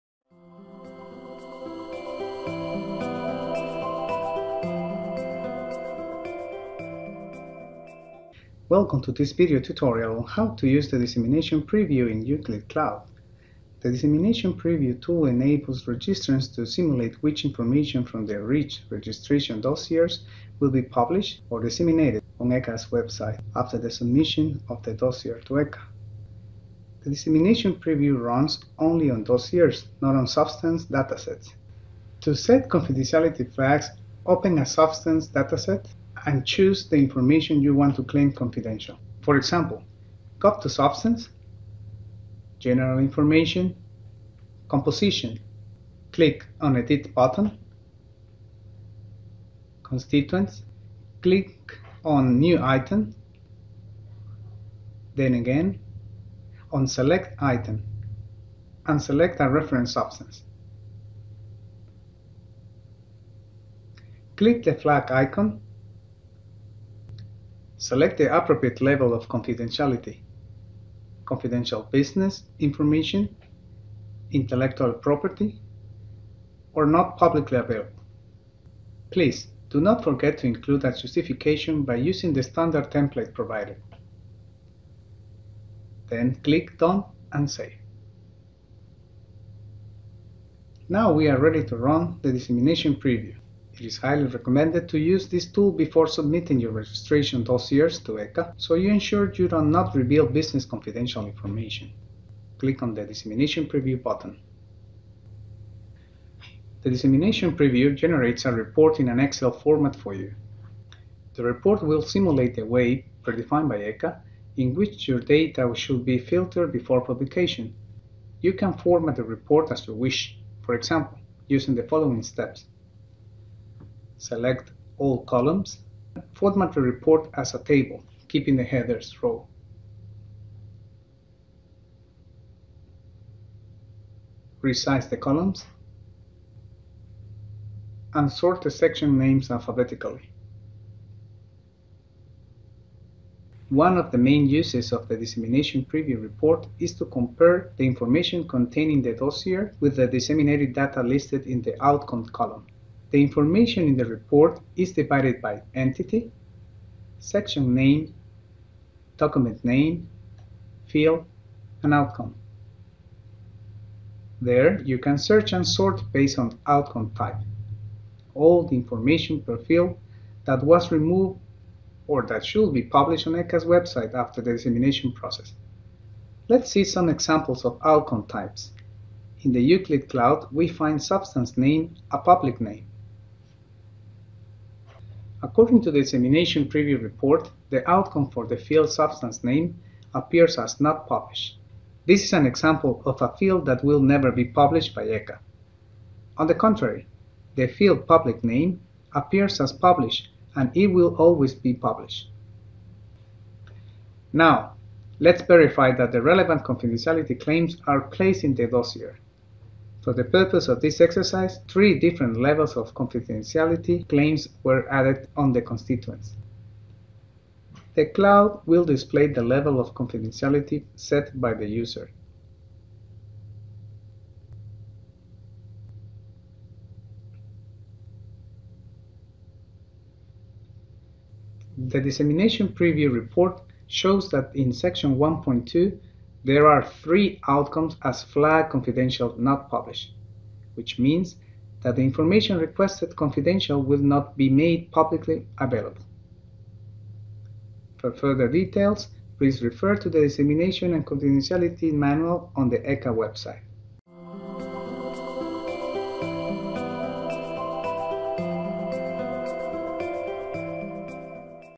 How to use the dissemination preview (web interface)
The dissemination preview tool allows you to simulate what information from your REACH registration dossiers will be published, or disseminated, on our website, after you have submitted your dossier to us.
This video shows how to use the tool in IUCLID using the web interface.